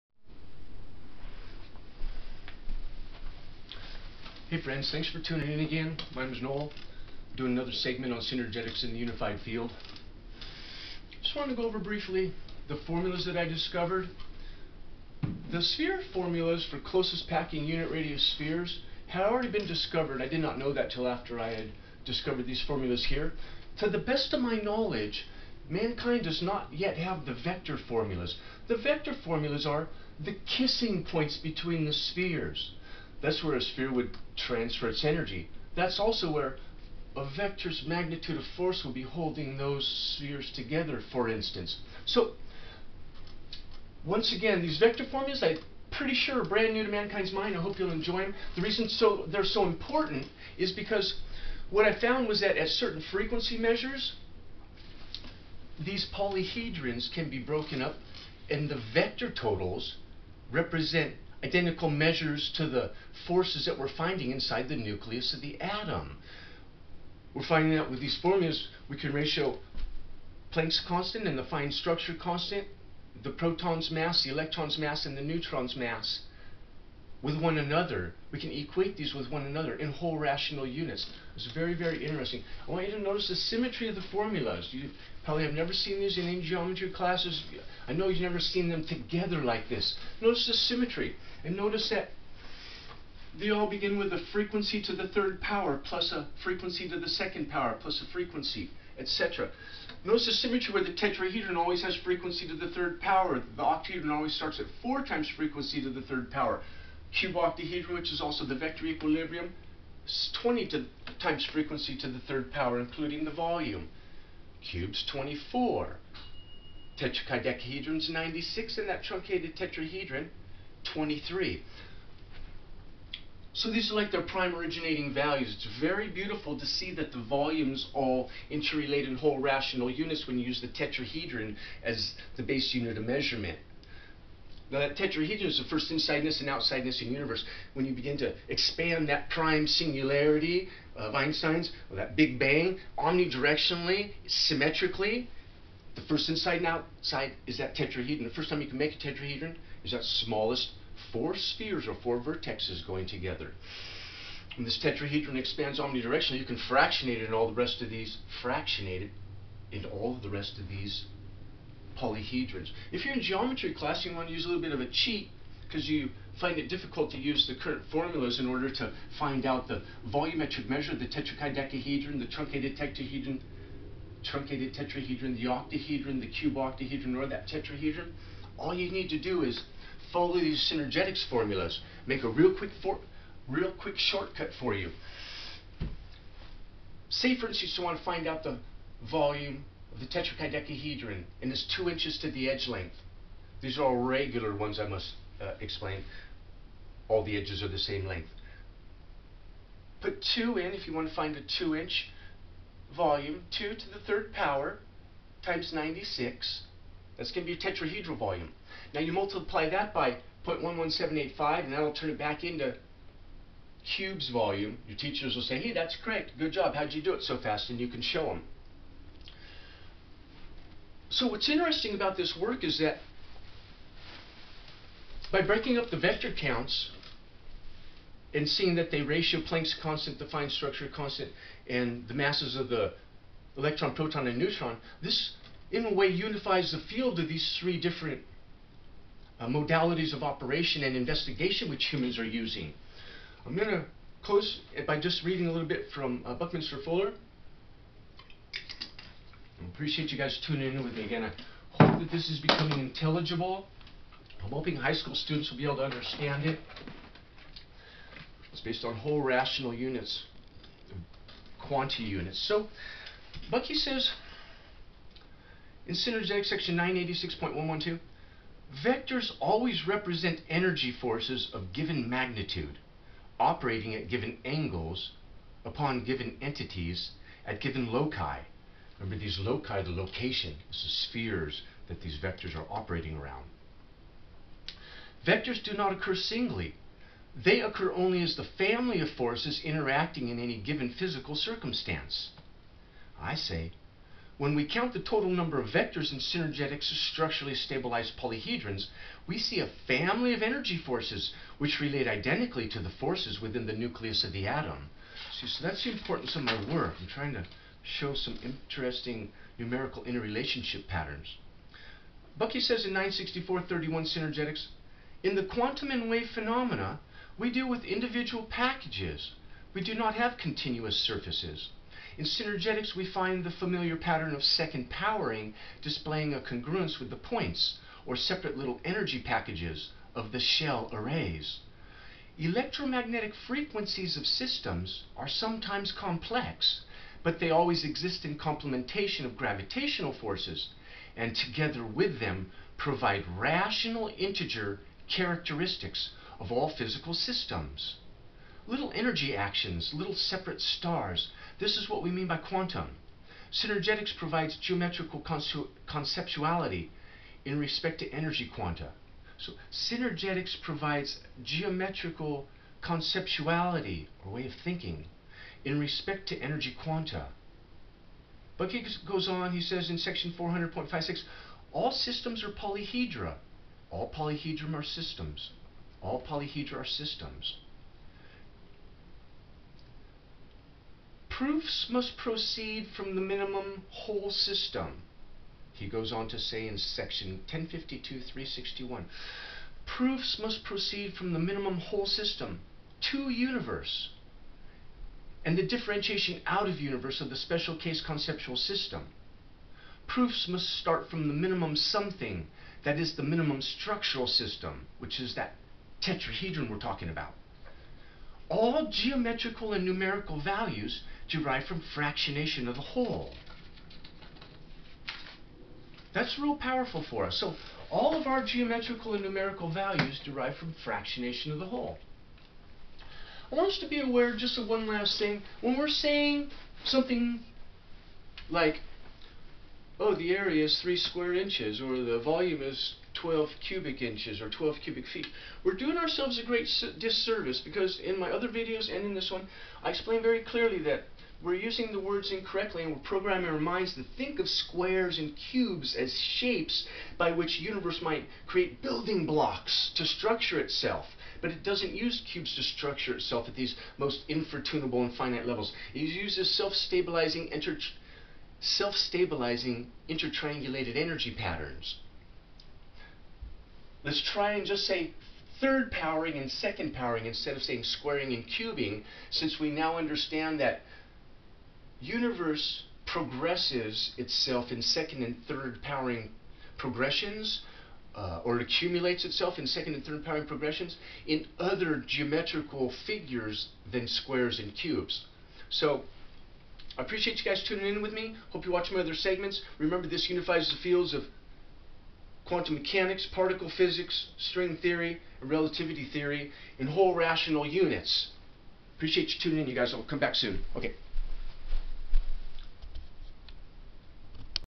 The Unified Field: Synergetics and Nuclear Structure: Part 4
In this video I discuss the formulas I discovered for closest packing unit radius spheres and their vectorial interconnections or 'kissing points'. I also speak about how these vector totals replicate the numerical inter-relationship patterns which exist within the nucleus of the atom.